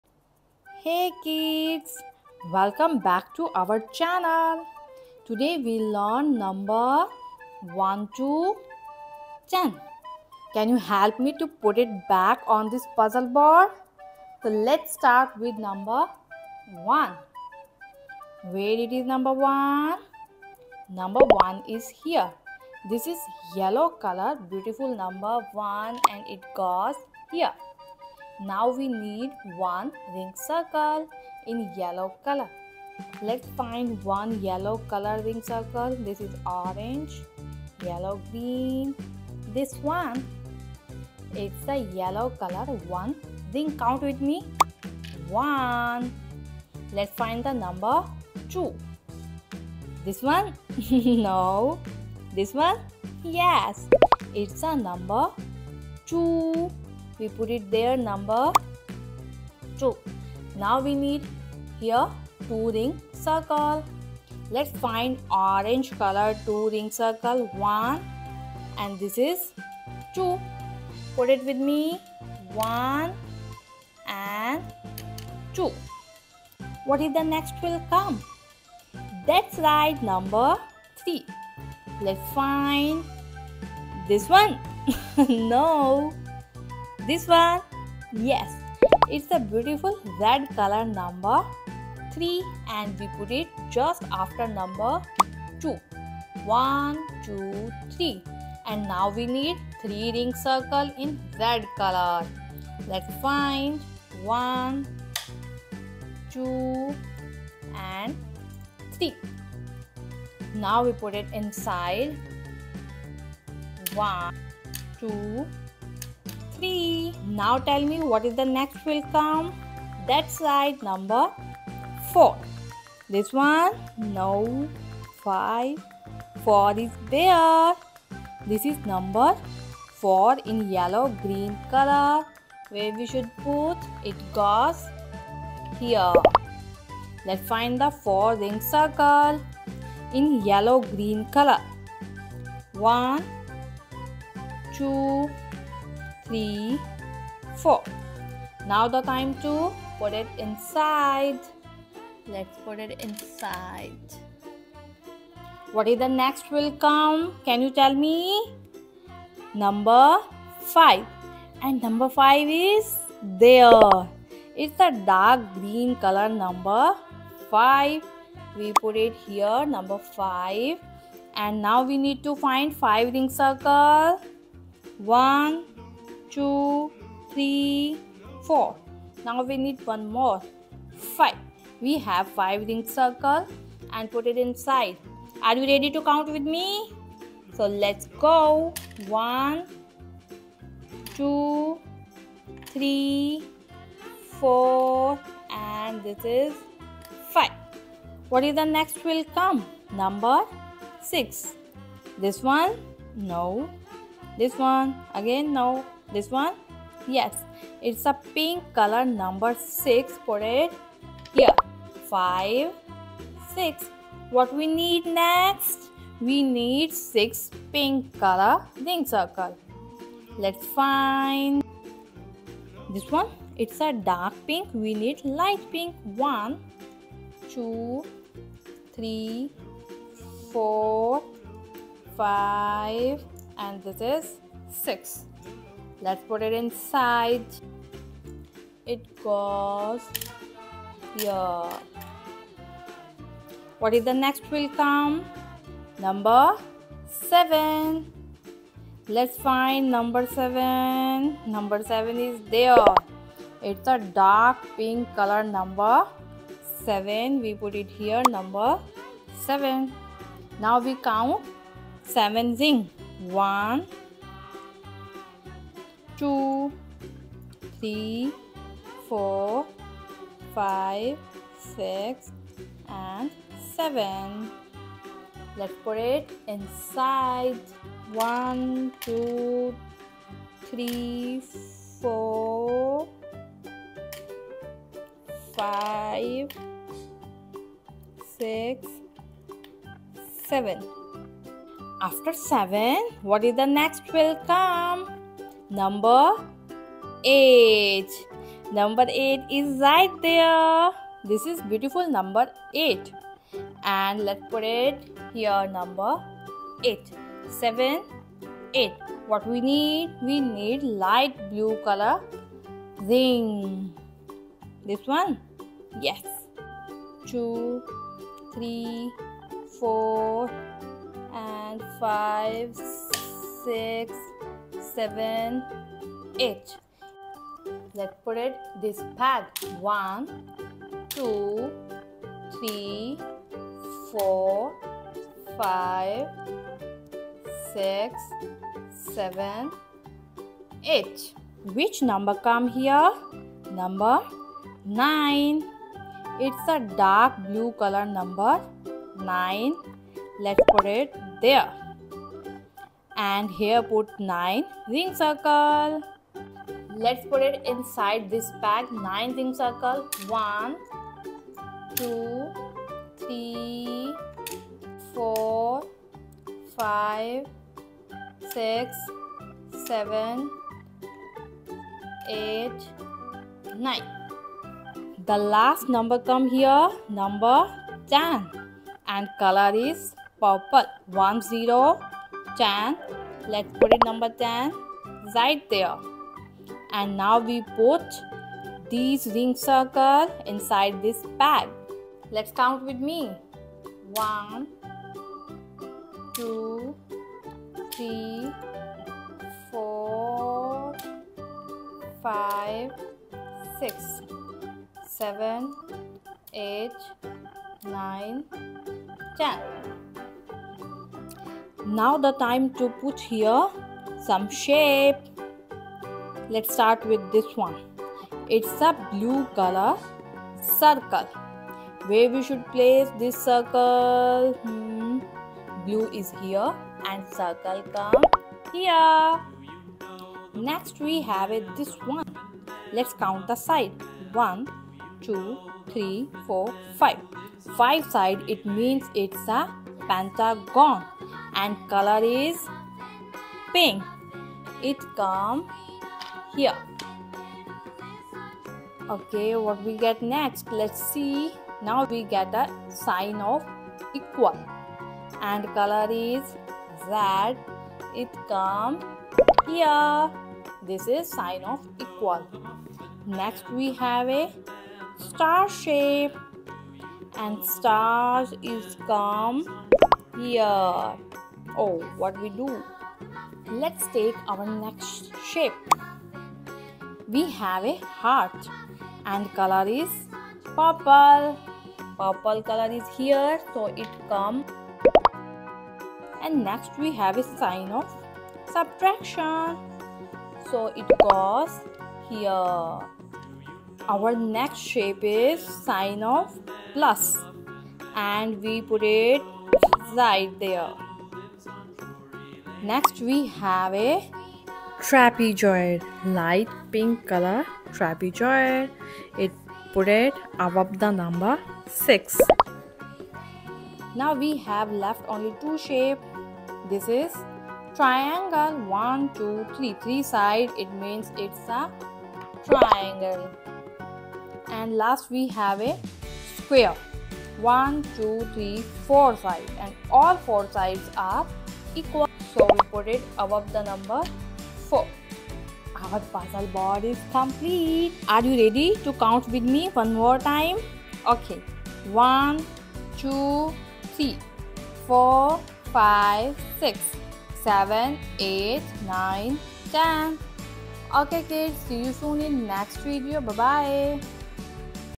Numbers and Counting Puzzle For Kids | Numbers Fun For Preschool Toddler
Let’s learn 1 to 10 counting and explore 10 fun shapes in this colorful and playful video made  for kids ,toddlers and preschoolers |

🌟 First, we count from 1 to 10 using bright and fun visuals
🎯 Then, for each number, we place that many circle rings on a peg
🔷 Finally, we introduce 10 different shapes like circle, square, triangle, and more |

This interactive video helps build:

Early math skills (1-10 counting)

Fine motor skills (placing rings)

Shape recognition

Color and object association

Perfect for toddlers, preschool, kindergarten, and early learners!